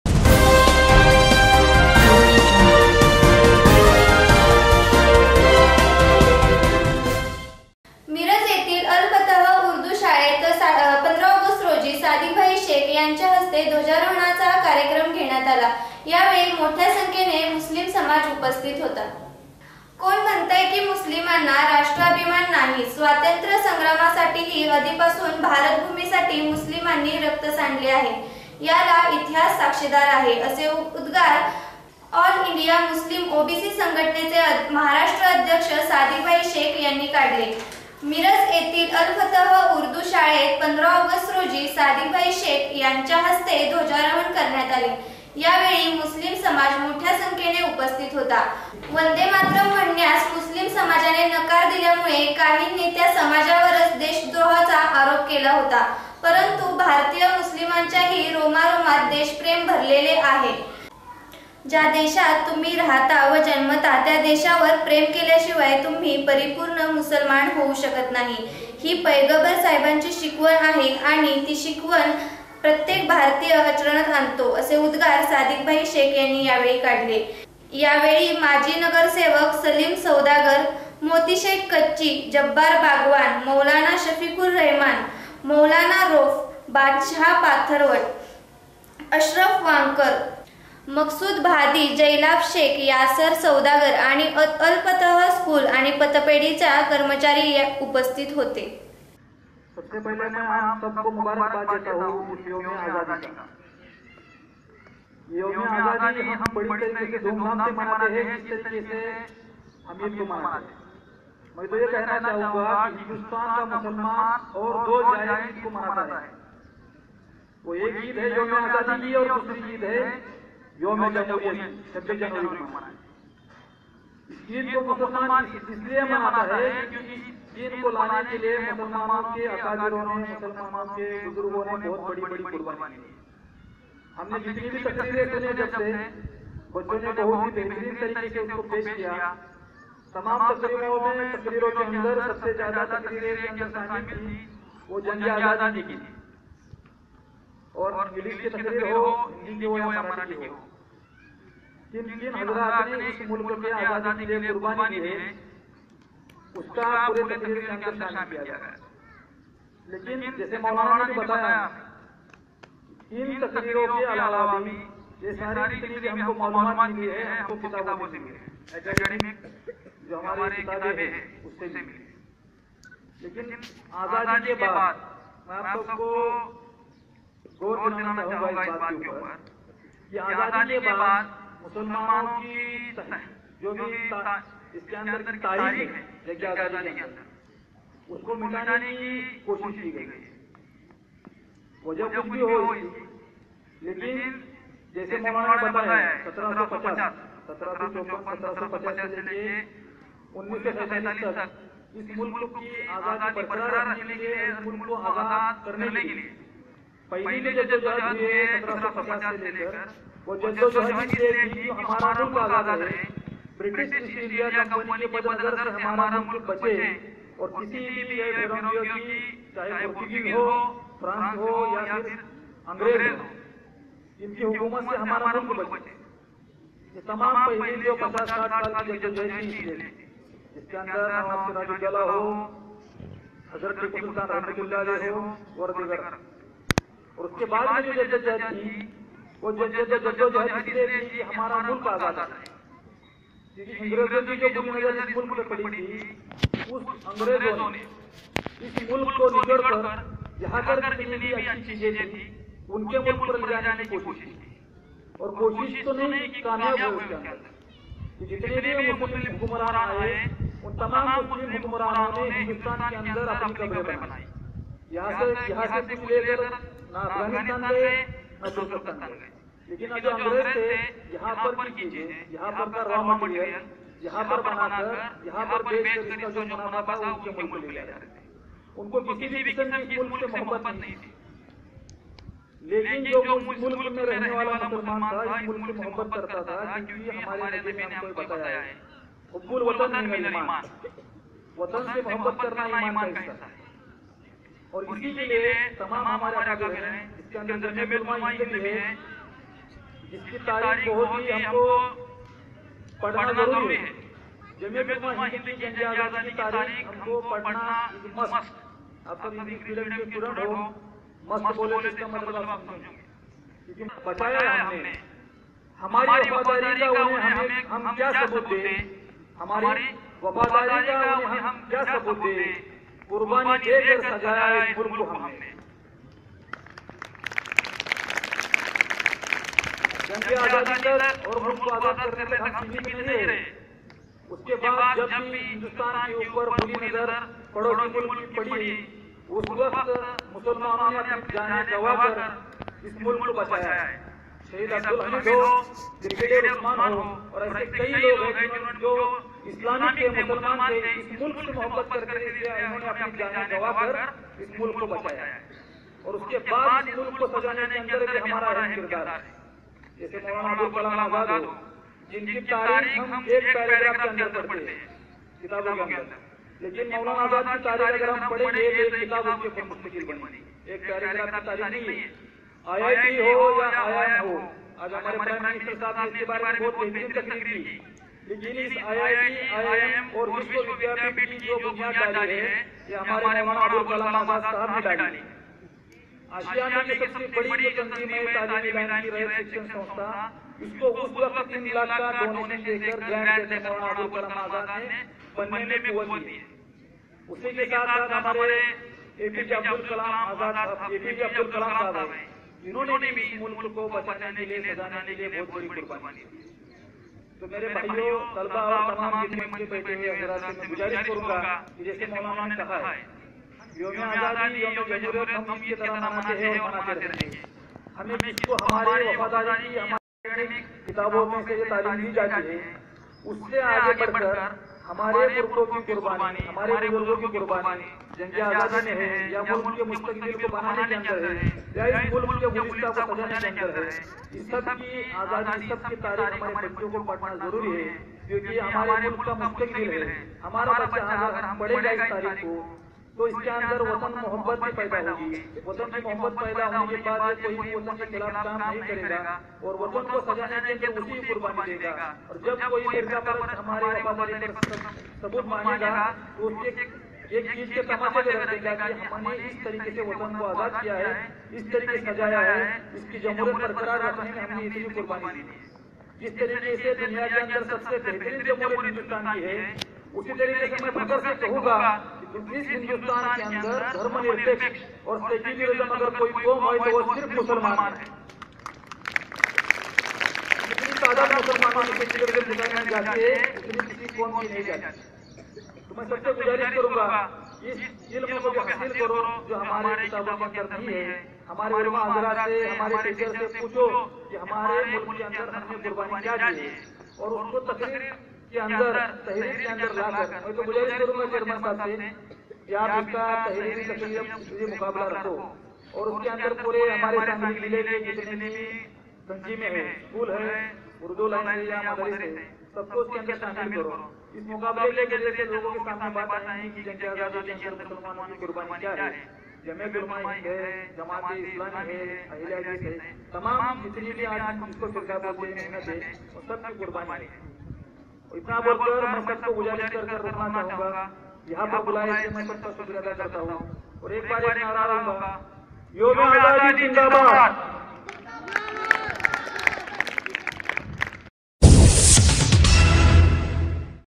sadikbhai shaikh yanchaya haste dhwajarohan.
मुस्लीमानी साजरा केला स्वातंत्र्य दिन तिरंगा फडकावून समाजाचा तथाकथित समाजाचा ठेका घेणाऱ्या मुस्लीम विरोधी लोकांना सडेतोड उत्तर.